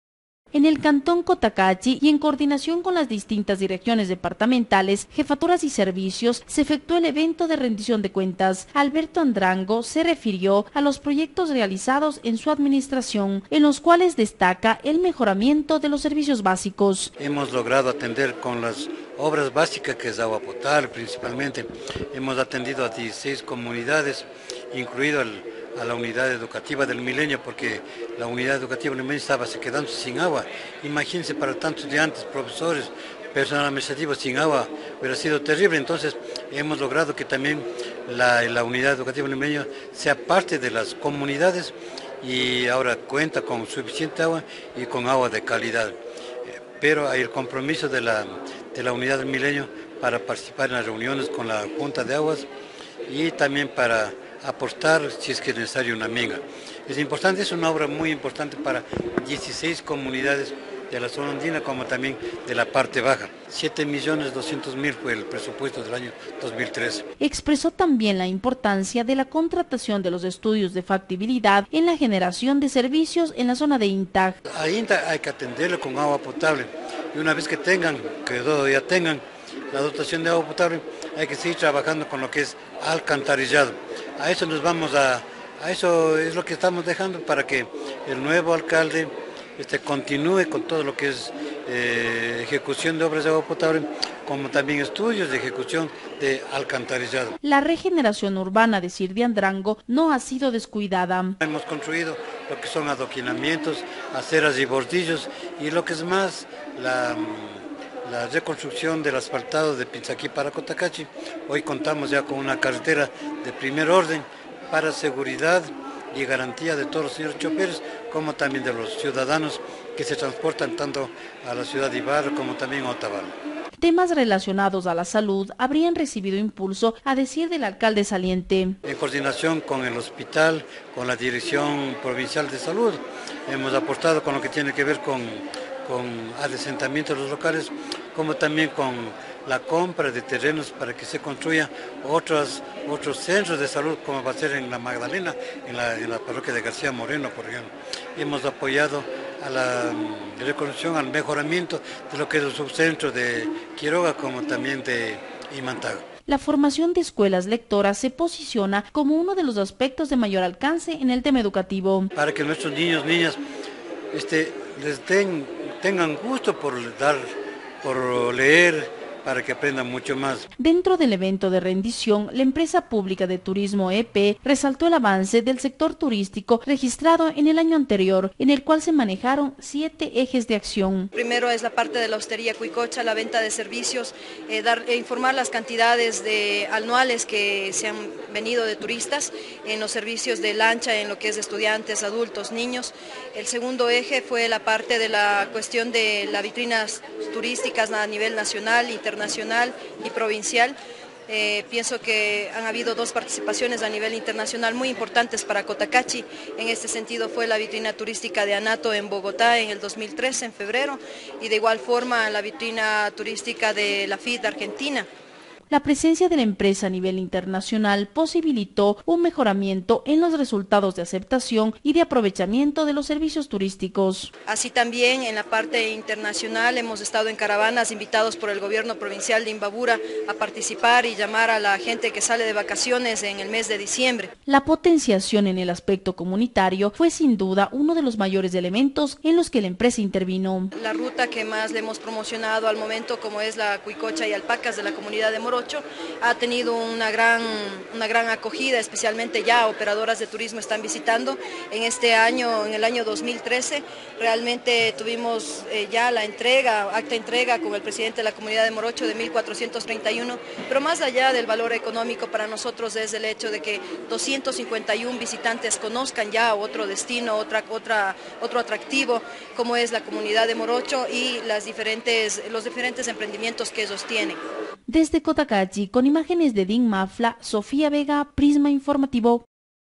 GAD Municipal de Cotacachi, rindió cuentas. (Noticias Ecuador)
En el cantón Cotacachi, y en coordinación con las distintas direcciones departamentales, jefaturas y servicios, se efectuó el evento de rendición de cuentas. Alberto Andrango se refirió a los proyectos realizados en su administración, en los cuales destacó el mejoramiento de los servicios básicos.